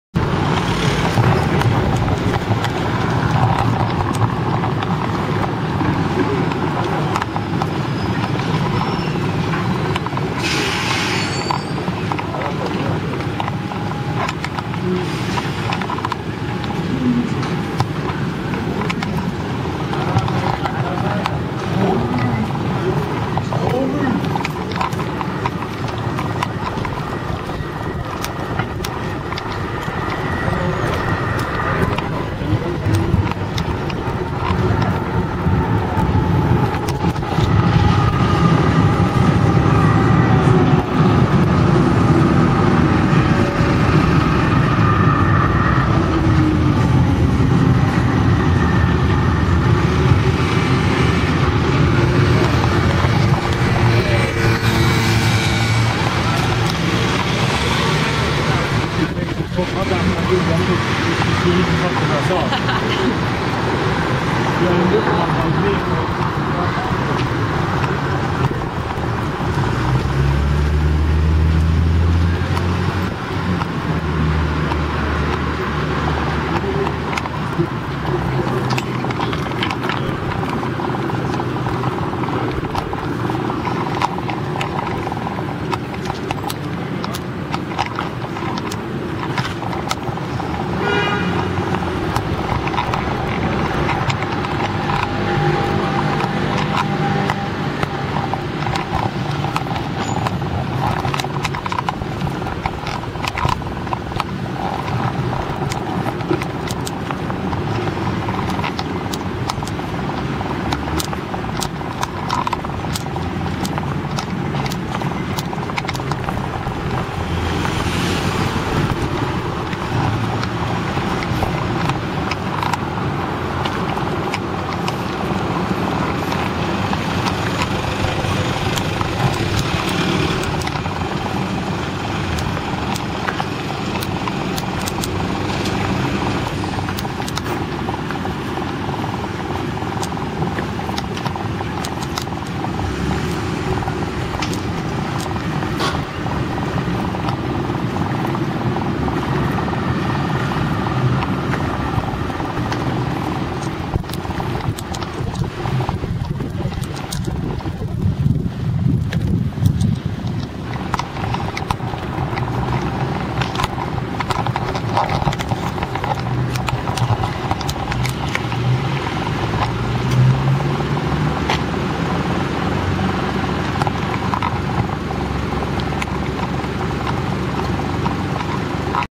Police van harassed innocent victim before Terrorist at headquarter of the Norwegian government
Before Terrorist police van often harassed the innocent victim at the same area, Anders Behring Breivik,  bombing of the headquarter of the Norwegian government and the killing of at least 68 youth at the Labour Party youth camp on Friday,